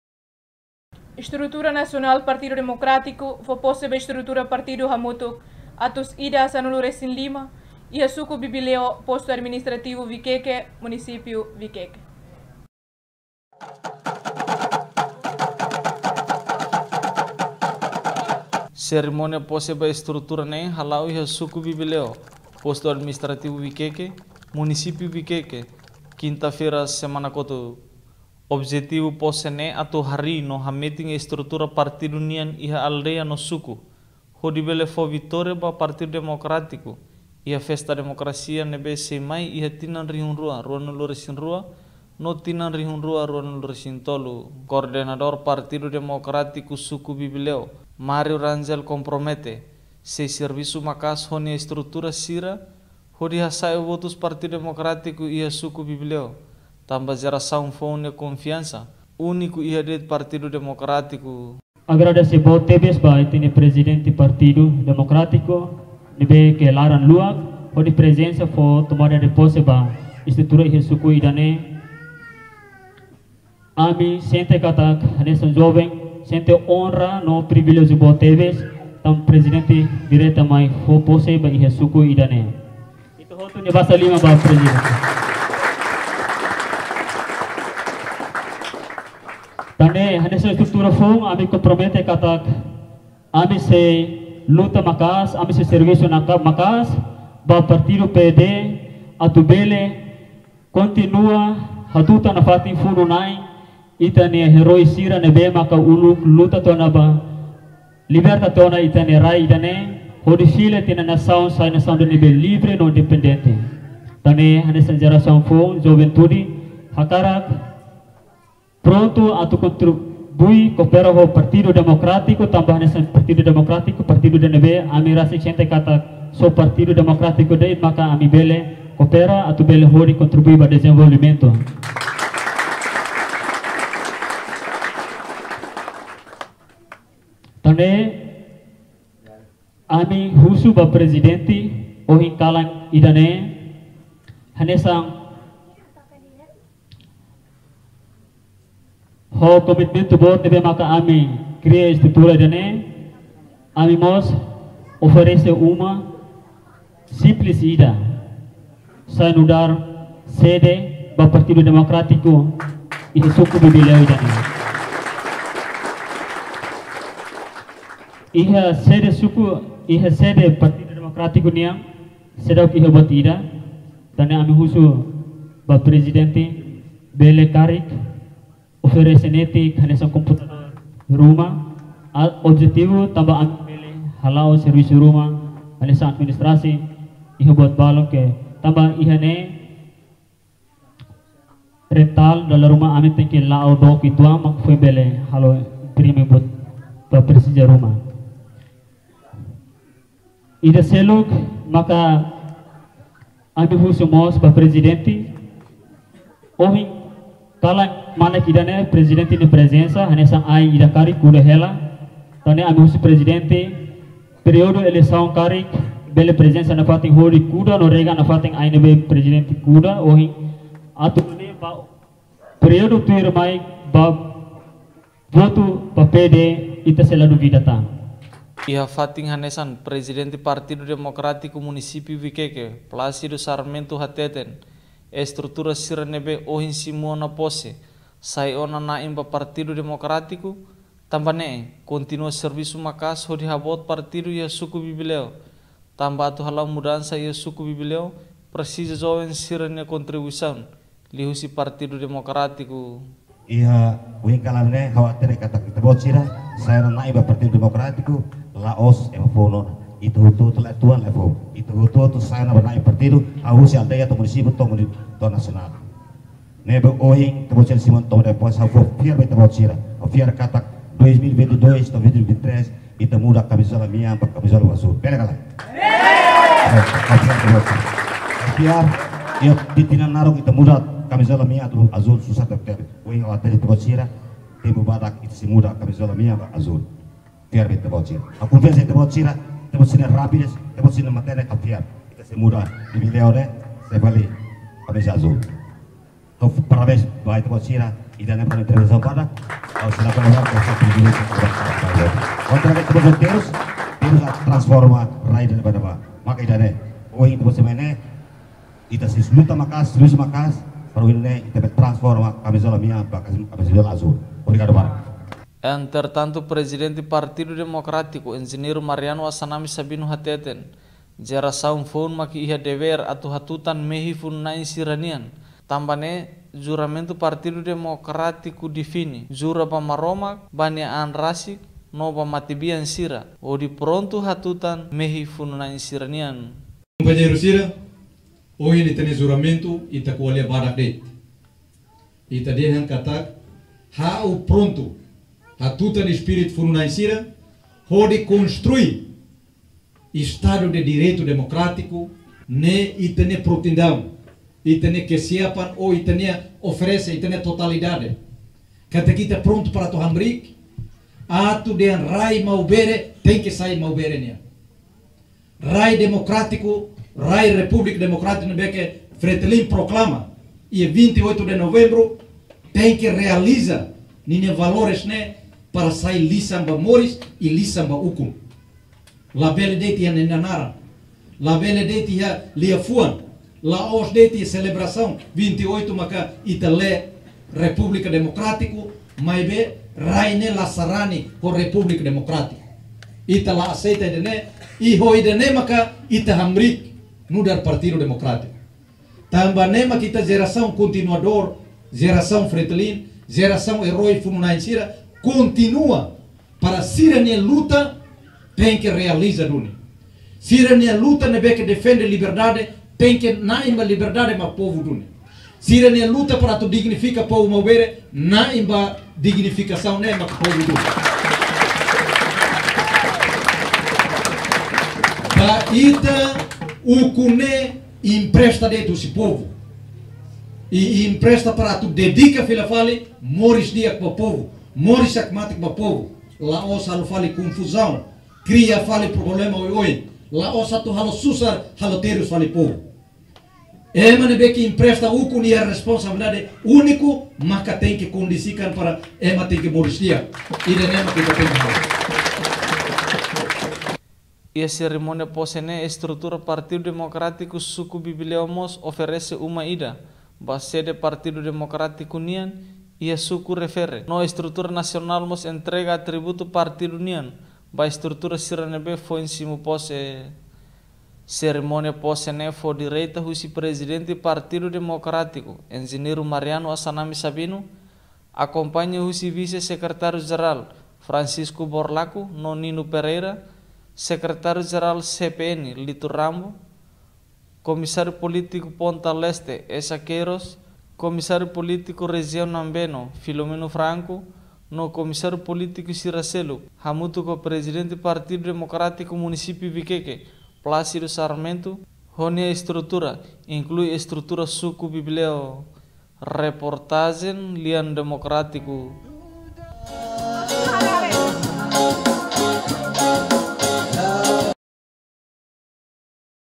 Tomada de Pose Estrutura PD Suku Bibleo Postu Administrativu Viqueque Hamutuk Ema Nain 115.
Konsolidasaun no Tomada de Pose Estrutura PD Suku Bibleo, Postu Administrativu Viqueque Munisipiu Viqueque Hamutuk Ema Nain 115